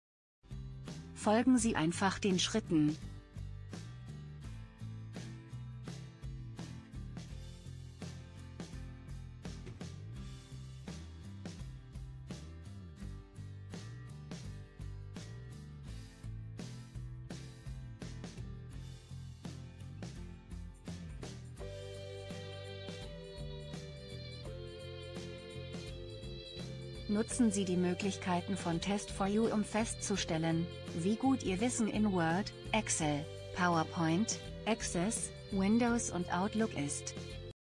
Verfassen und senden Sie eine neue Nachricht an mit dem Betreff Bild. Hängen...
Hängen Sie dabei die Datei image.jpg vom Ordner TEST4UFolder an, der sich auf dem Desktop befindet. Möchten Sie mehr. Laden Sie das TEST4U demo herunter